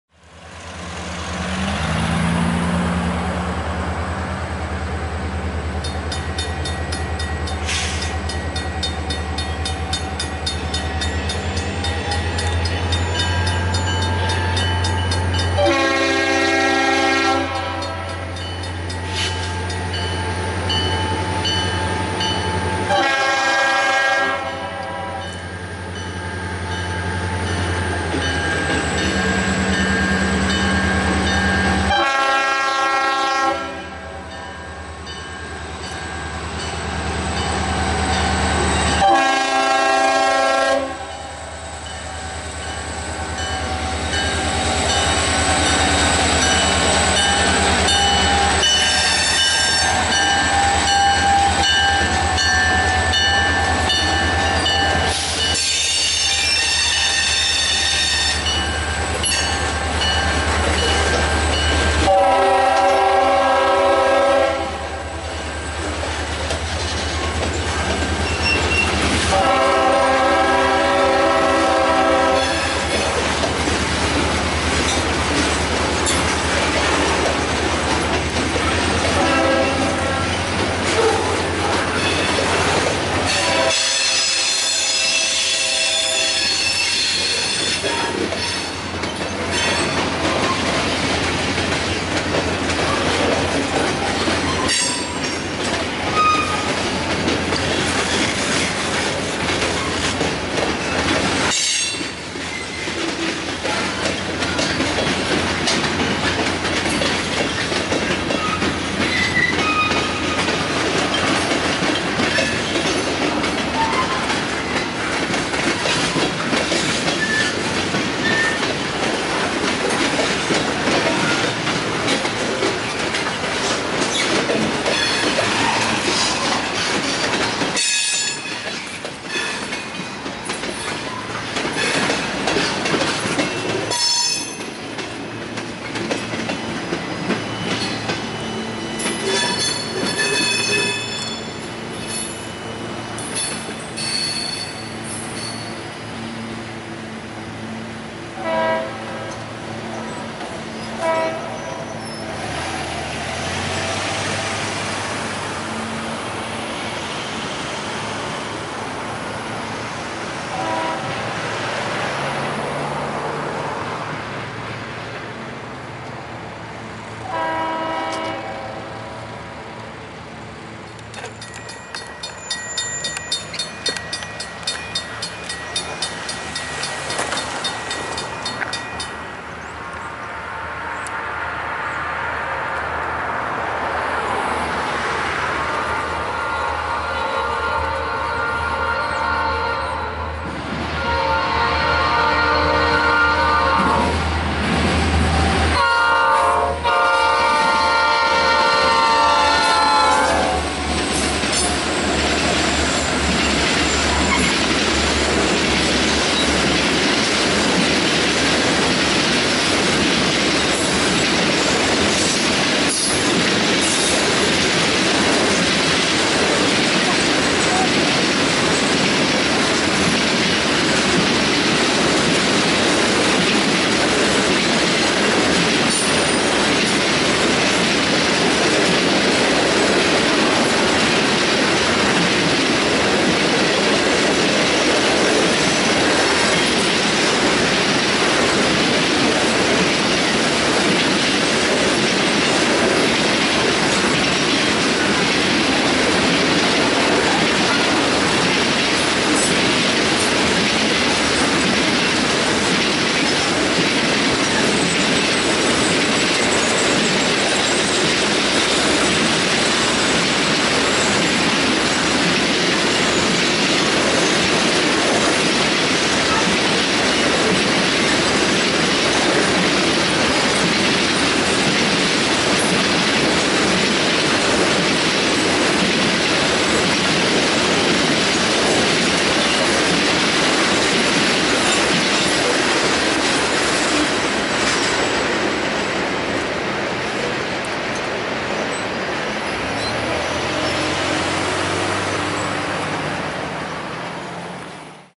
Town St. Grade Crossing (Fostoria, OH) 10/2/14
This crossing is on the NS Fostoria District.

Northern Signal
Bell:  Safetran Mechanical Bell
Lights:  2 pairs of 8"x20" Safetran incandescents
Gate lights:  4" NEG incandescents
Gate Mechanism:  Safetran

Southern Signal
Bell:  None
Lights:  3 pairs of 8"x20" Safetran incandescents
Gate Mechanism:  Safetran